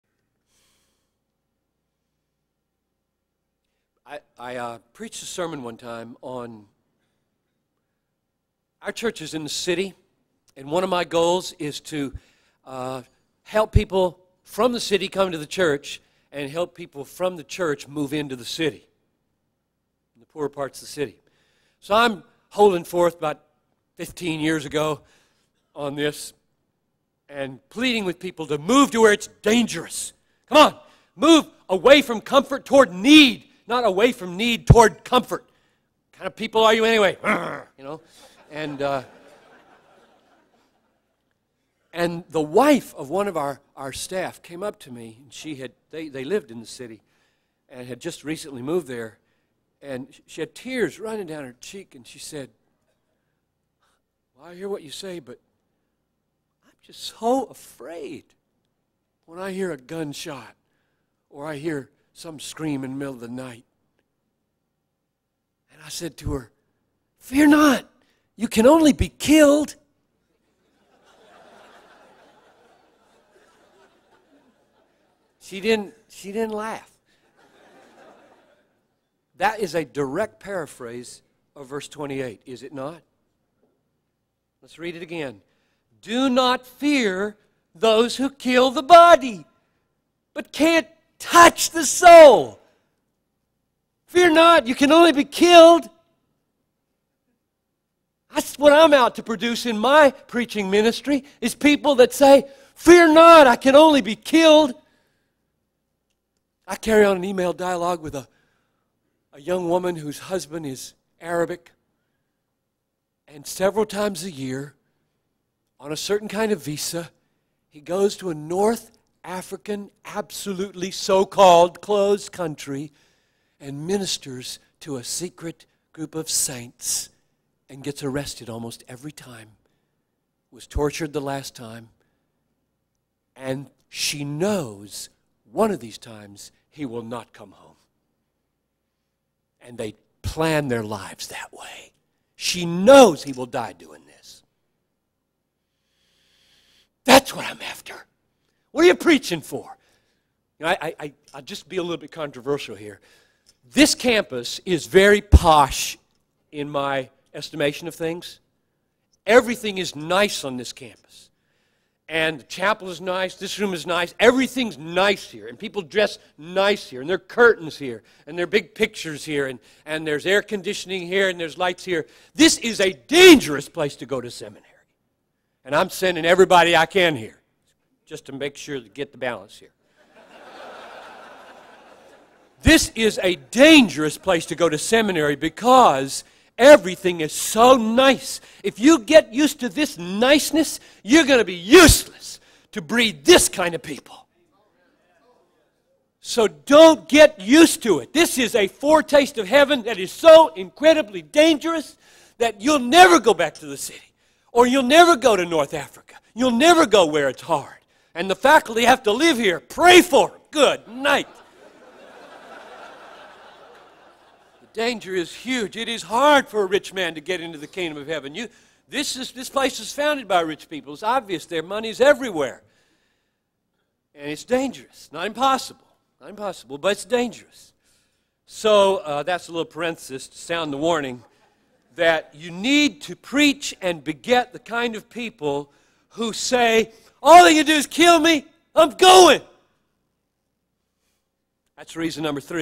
John Piper: Fear not! You can only be killed! - Dying so Muslims may be saved - Dying to comforts
Matthew 10:28: "And do not fear those who kill the body but cannot kill the soul. Rather fear him who can destroy both soul and body in hell."

John Piper gave advice to a lady in his church who had moved into the city. She had tears in her eyes and told him how scared she was when she heard gunshots or screaming in the middle of the night. His advice? - "Fear not! You can only be killed!"

Then he told this story: "I carry on an email dialogue with a young woman whose husband is Arabic, and several times a year, on a certain kind of visa, he goes to a North African, absolutely so called "closed country" and ministers to a secret group of saints and gets arrested almost every time, was tortured the last time, and she knows one of these times he will not come home. And they plan their lives that way. She knows he will die doing this. That's what I'm after! What are you preaching for!"

This clip was part of a talk John Piper gave at the Southern Baptist Theological Seminary in 1999 called "A Call For Courage In Christian Ministry"